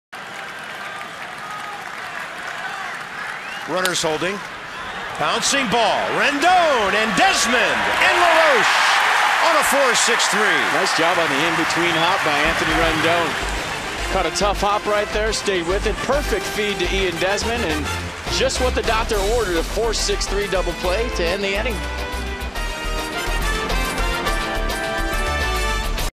2013/07/02 Rendon turns two
MIL@WSH: Rendon turns tough double play in the fourth